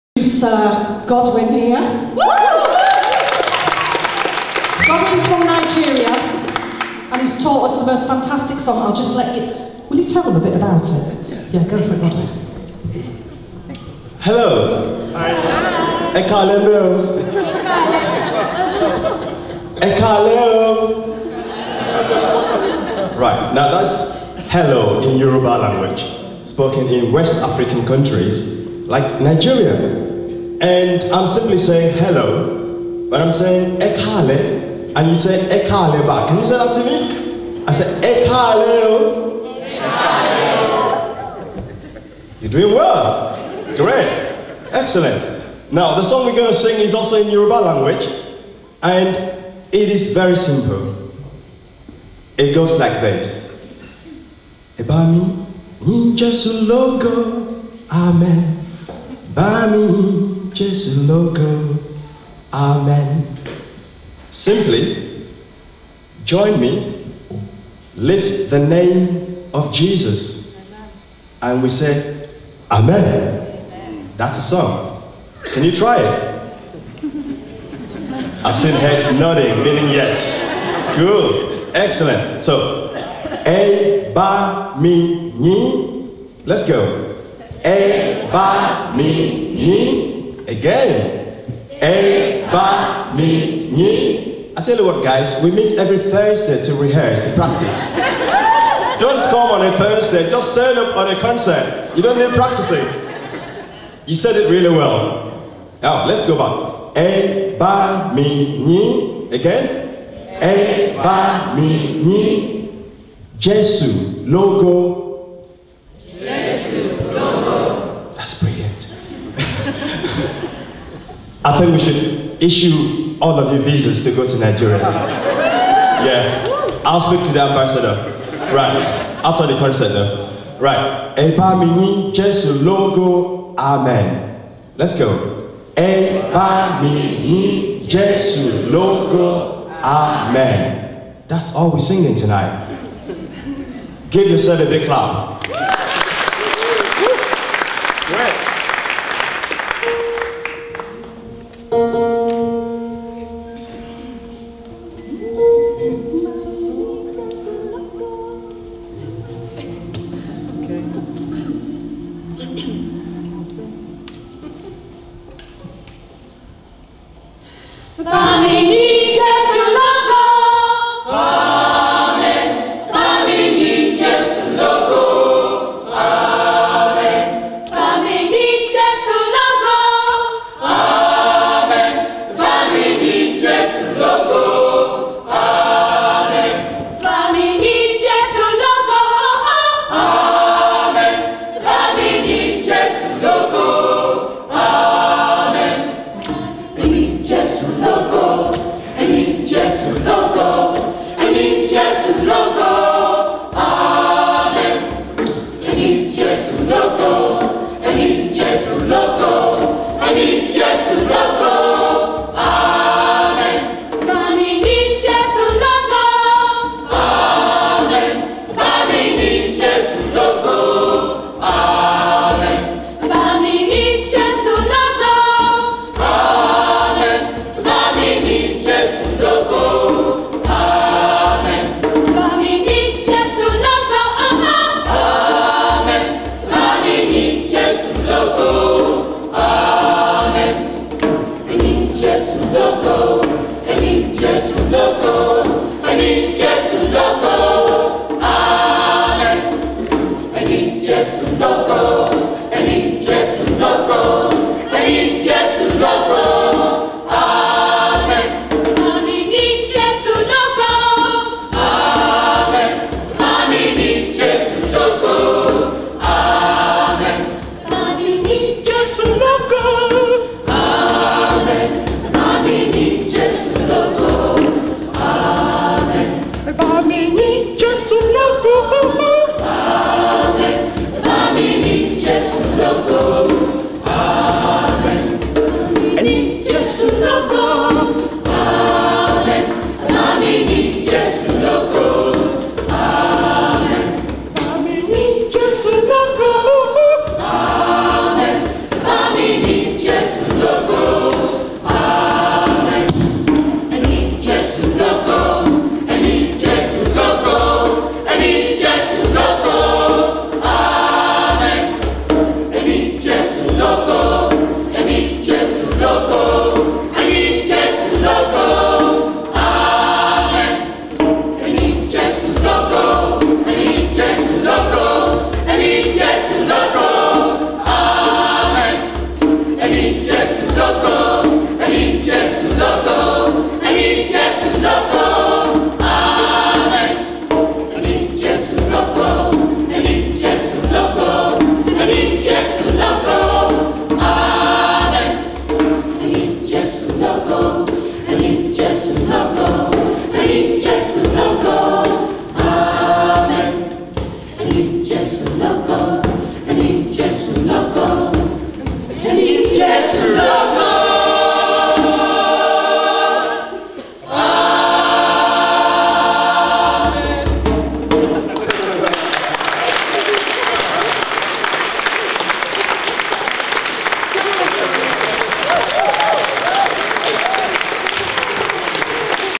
Godwin
Hot Gospel performing A Nigerian Song at All Saints Church Hessle Hull. Directed by Godwin Abot